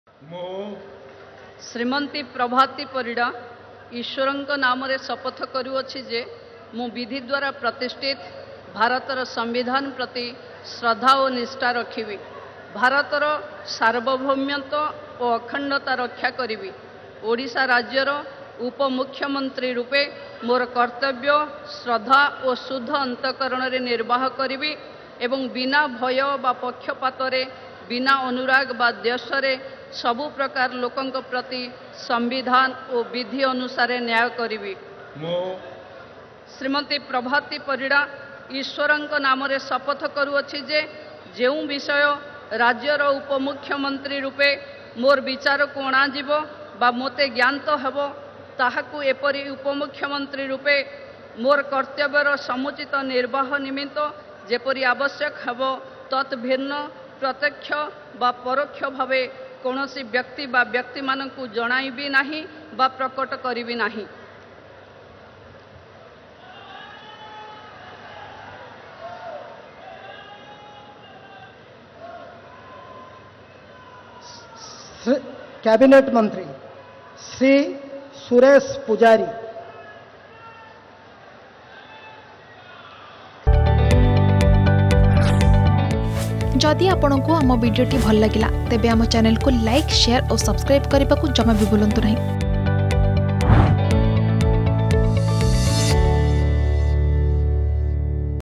ଉପ ମୁଖ୍ୟମନ୍ତ୍ରୀ ଭାବେ ଶପଥ ନେଲେ ପ୍ରଭାତୀ ପରିଡା || Oath Taking Ceremony
ଉପ ମୁଖ୍ୟମନ୍ତ୍ରୀ ଭାବେ ଶପଥ ନେଲେ ପ୍ରଭାତୀ ପରିଡା